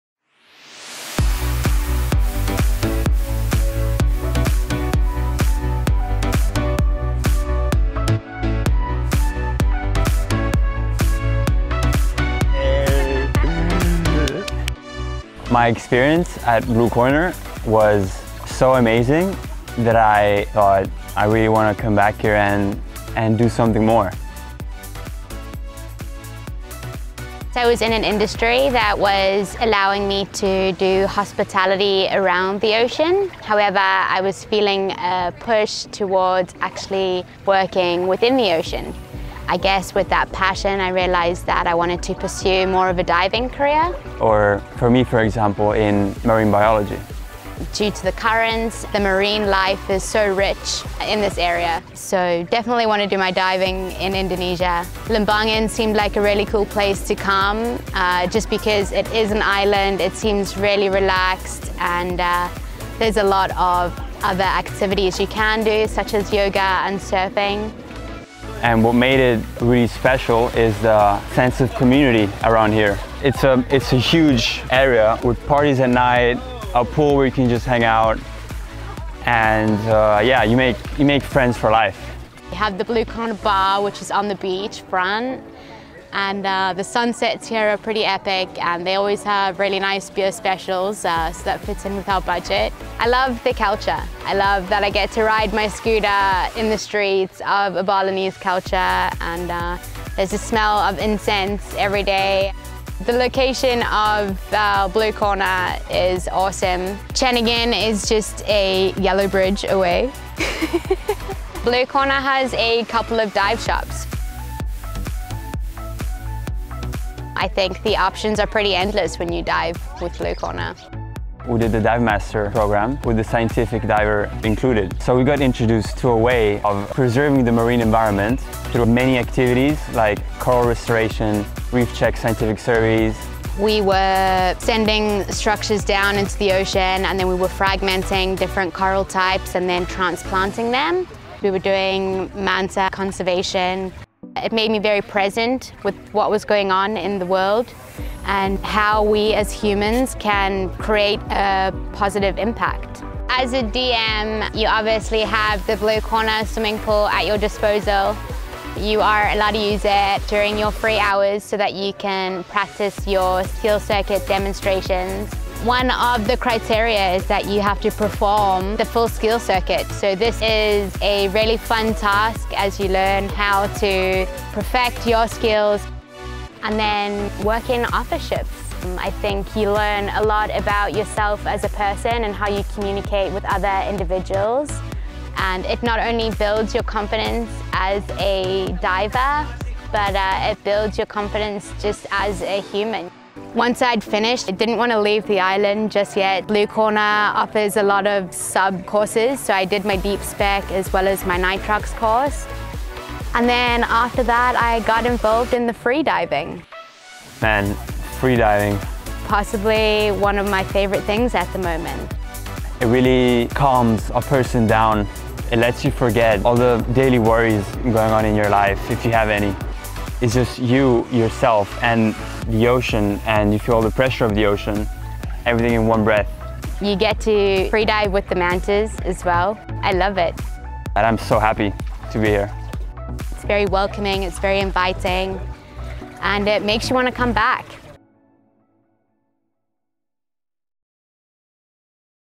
Divemaster Diaries with Blue Corner Dive
The Blue Corner PADI Divemaster program is renowned for being unique and fun! Hear it firsthand from two of our most recent candidates who got involved in literally everything Blue Corner Dive has to offer.

From making an impact in the Scientific Diver program to becoming obsessed with Freediving, learn about their life changing experience both above and below the surface of Nusa Lembongan, Bali.